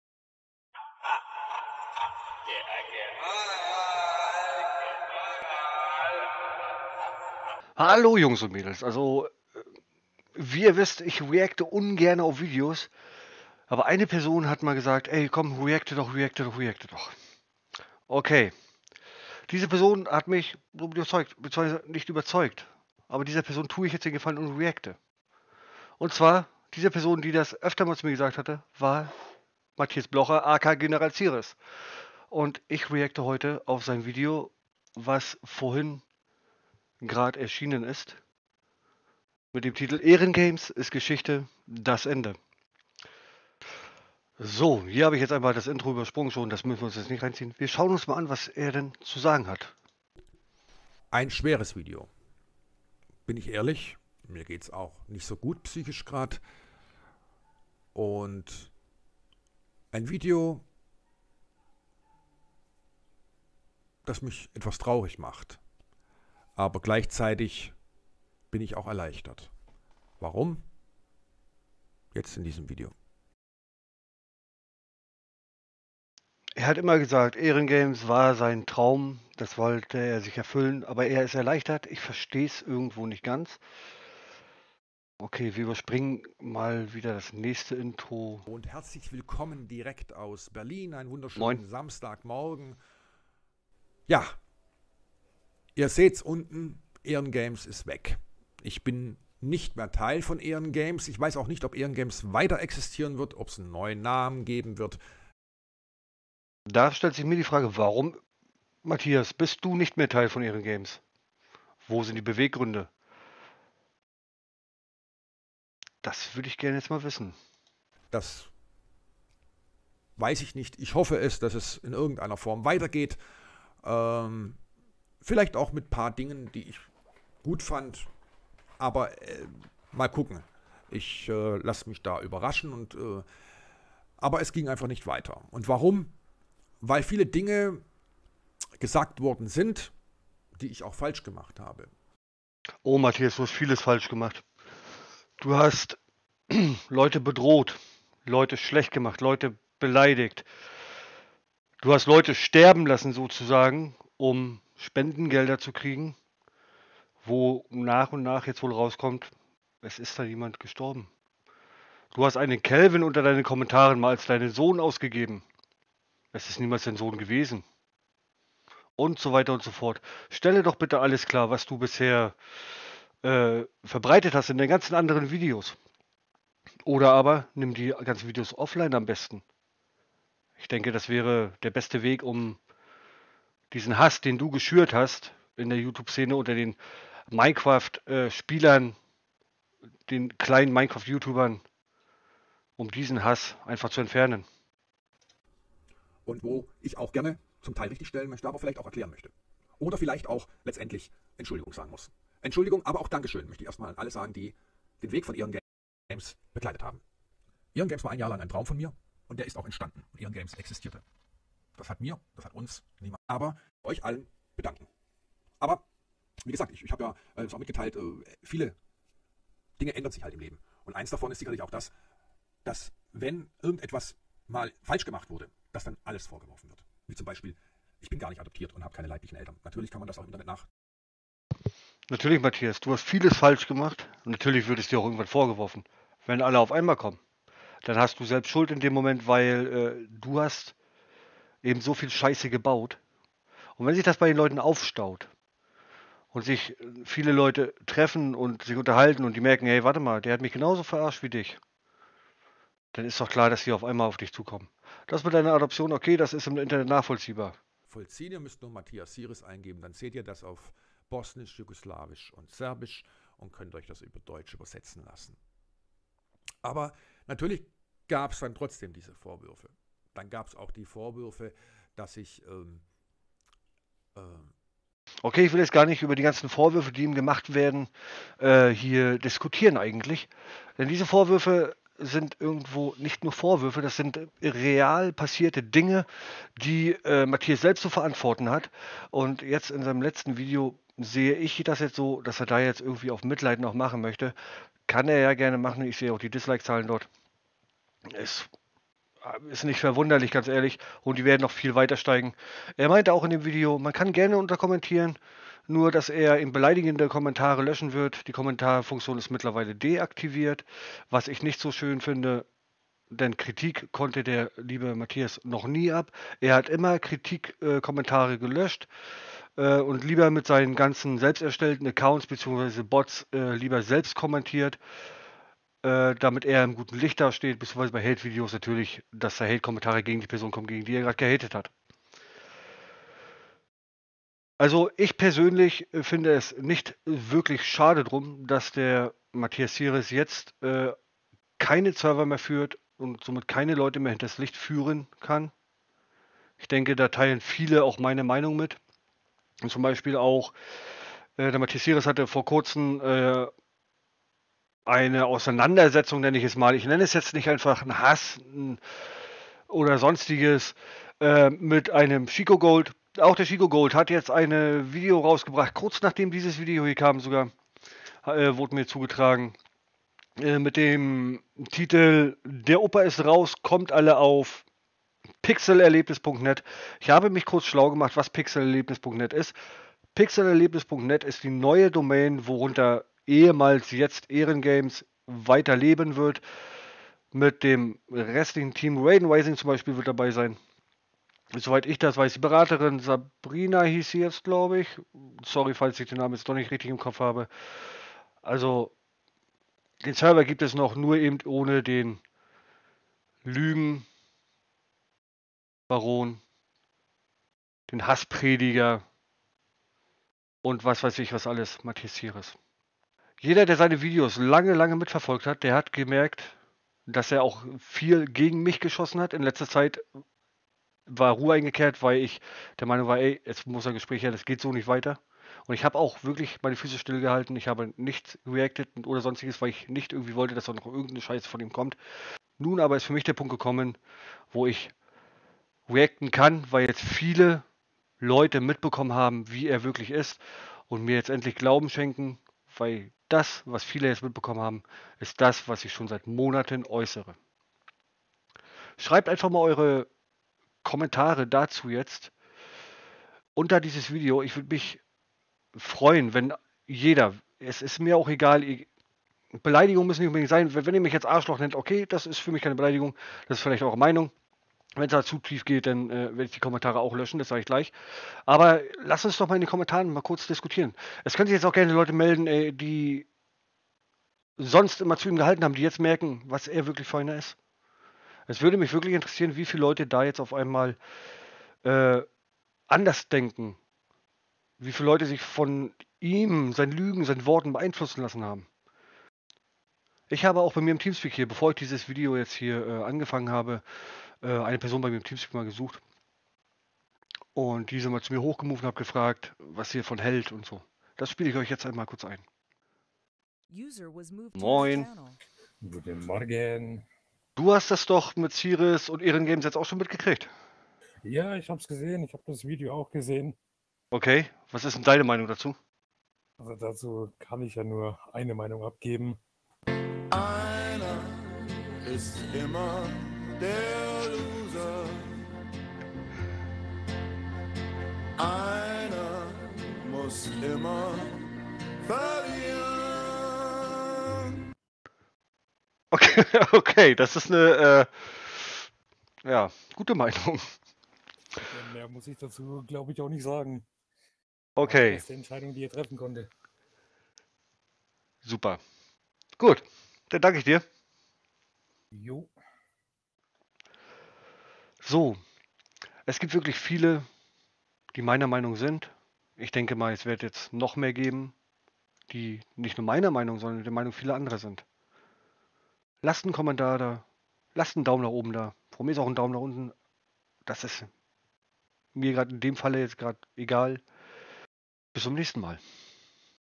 Es war an der Zeit !!! | Das Ende von General_Ziris und Ehrengames !!!
Wie ist eure Meinung zu dem Abgang von Mathias Ziris (Blocher) welcher längst überfällig war !

Ehemals Ehrengames findet Ihr nun unter PixelErlebnis.net welcher nun von zumindest  @Raiden_Rising TV  und auch dem Rest des ehemaligen Ehrengames Teams geführt wird !!!

Würde mich über Donations freuen um mein Equipment zu verbessern und die Qualität weiter zu steigern: